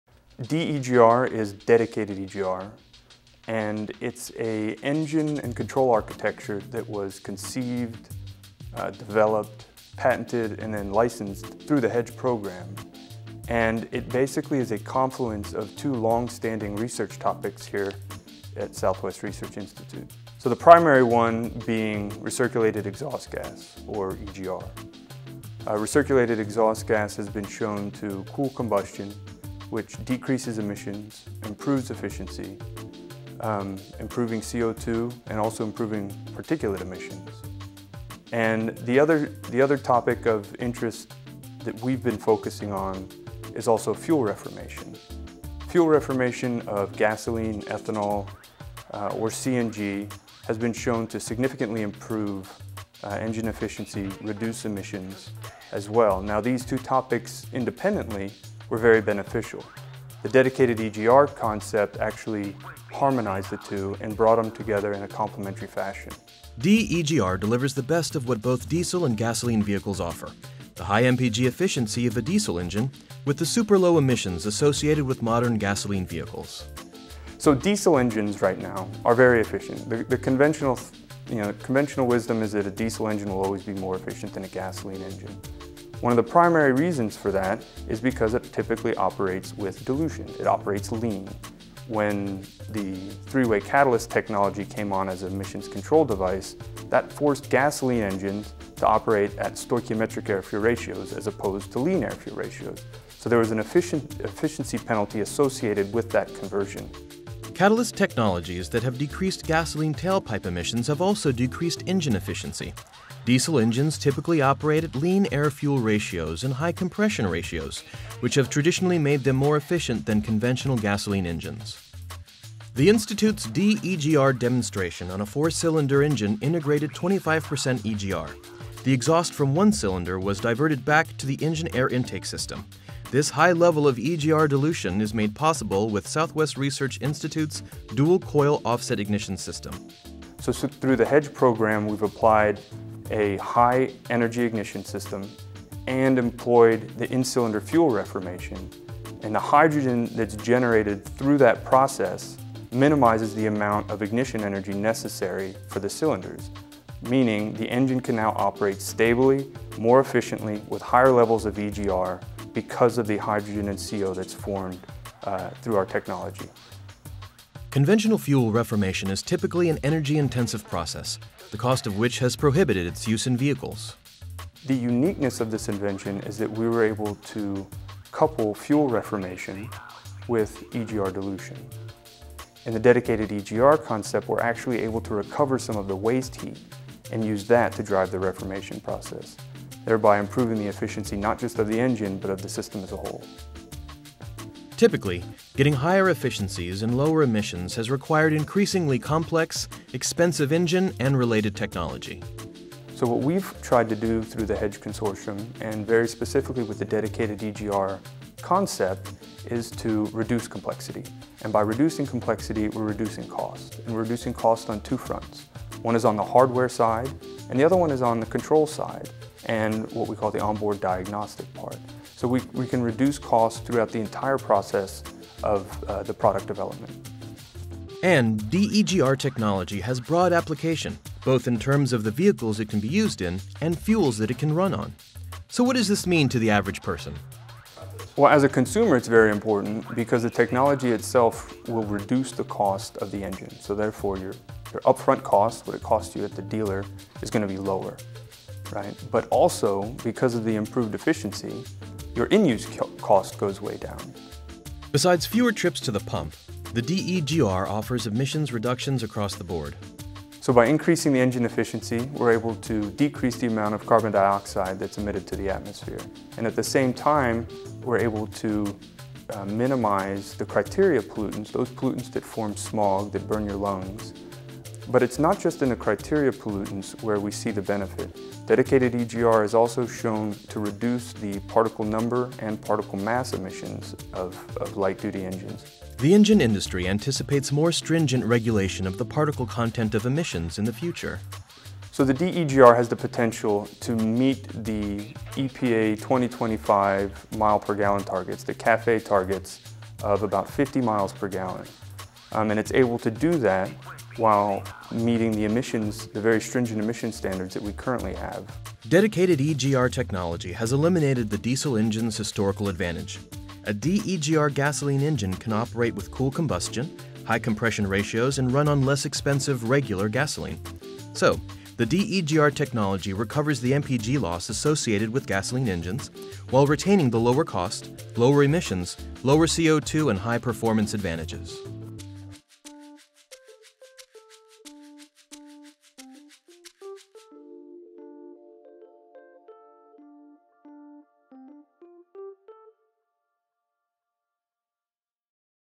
D-EGR
Southwest Research Institute:

The Dedicated Exhaust Gas Recirculation (D-EGR™) engine is a cost-effective, high-efficiency, ultra-low emissions internal combustion engine SwRI developed for automotive and off-highway applications. It combines the efficiency improvements of recirculated exhaust gas with fuel reformation in a closed system where combustion heat, normally ejected to the atmosphere, is used to reform the fuel.